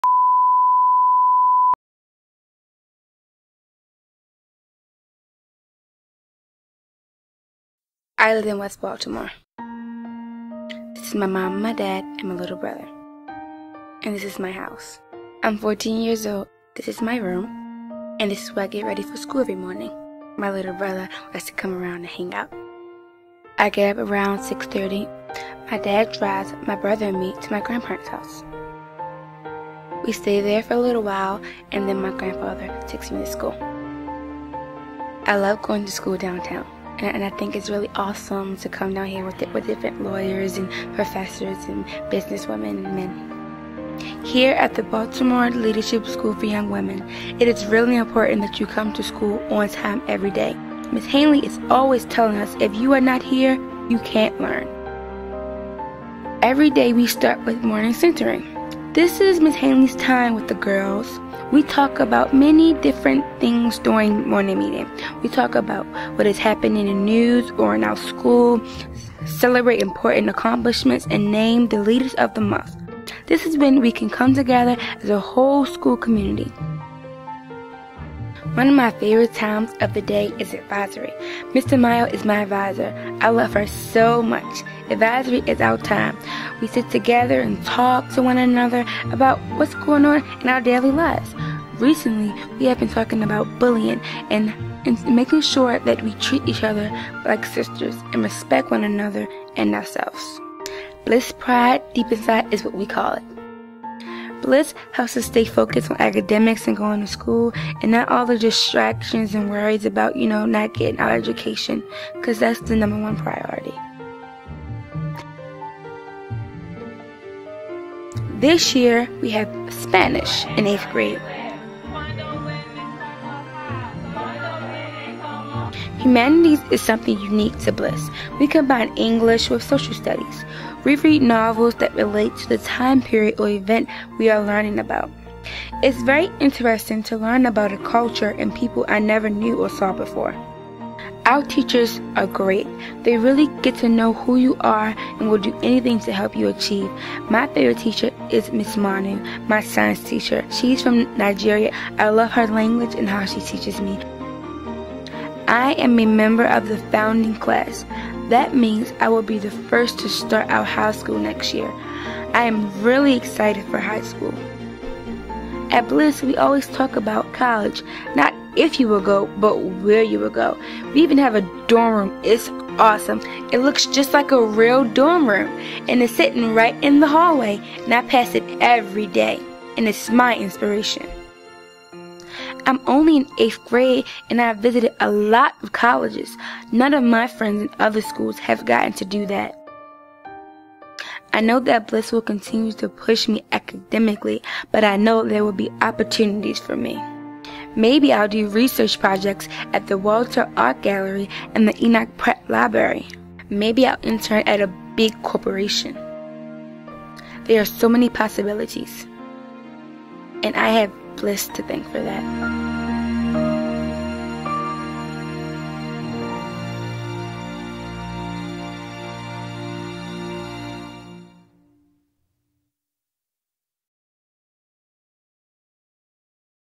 A Day in the Life of a BLSYW Girl!
Short video personal testimony from one of the 2016 founding graduating class of the Baltimore Leadership School for Young Women (BLSYW).  BLSYW is a college preparatory, all girls, charter school serving grades 6-12.  Our goal is simple -- to create a private school environment for public school kids.  At BLSYW we expect nothing less than a 100% graduation and college acceptance rate.  Our girls come to school and ask "What does BLSYW have in store for me today."  An investment in BLSYW is an investment in their future.